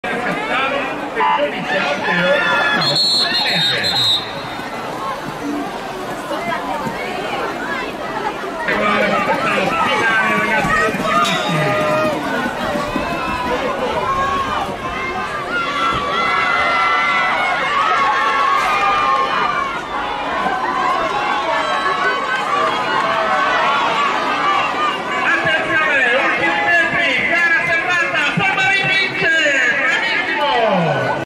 XVII Trofeo di Primavera - R12/M 200m sprint, finale
XVII Trofeo di Primavera, specialità "strada".
1 Aprile 2024, Paderno d'Adda (LC) - Italy.
Organizzazione: A.S.D. Padernese.
Categoria Ragazzi 12 Maschile.
200m sprint sprint. Finale.

00:00   finale, f.p.
00:08   finale

Risultati
1 377 MAZZI PAUL MARIE 310 POL. BELLUSCO 21.827
2 9 CAMPAGNA SIMONE 218 PATT.CARONNO PERT. 22.103
3 347 COCCO ALESSANDRO 3902 SPEEDYROLLER CASSANO 22.556
4 345 SCOLLO GIONA 3902 SPEEDYROLLER CASSANO 22.747
5 353 GIOLITTO MATTEO 1285 PATT.S.MAURO TO 22.945
6 344 BONACCORSO FILIPP CRISTIAN 3902 SPEEDYROLLER CASSANO 23.205